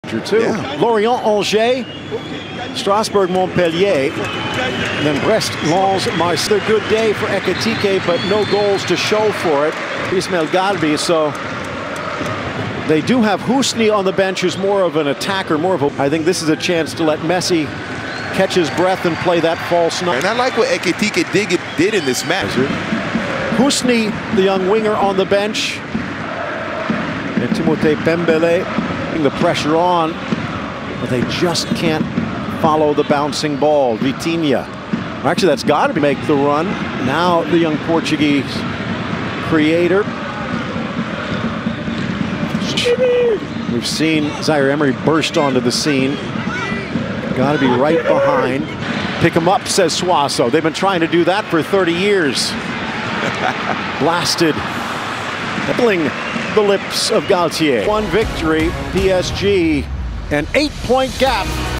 Ismaël Gharbi vs Toulouse (2/4/23)
Ismaël Gharbi is a young and promising player that is eligible to represent Tunisia, France, or Spain. He has represented both France and Spain at youth level. He has most recently played for the Spanish U19 team. He played 5 minutes as a substitute in this Ligue 1 match.

إسماعيل الغربي